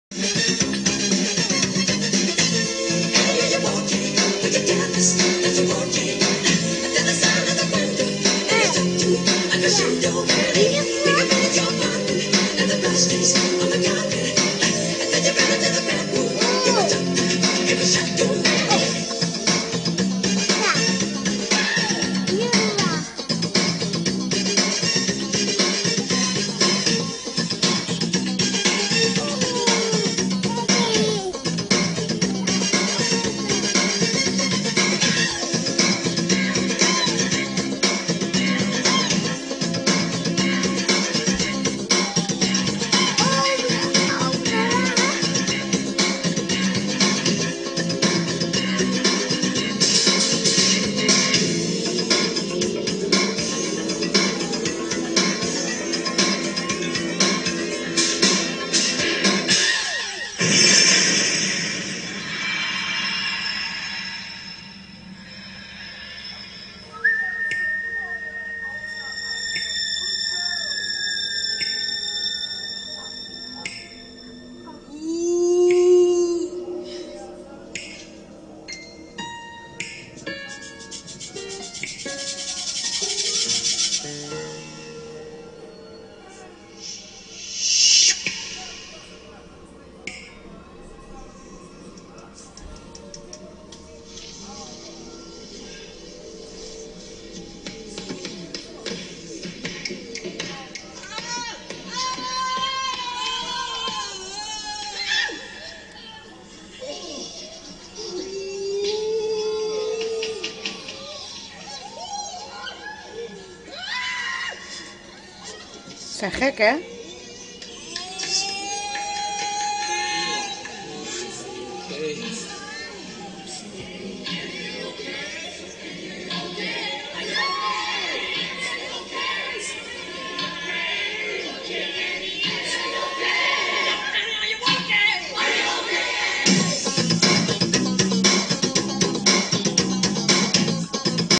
sem ziet het
hij was aan het dansen tot hij de camera zag.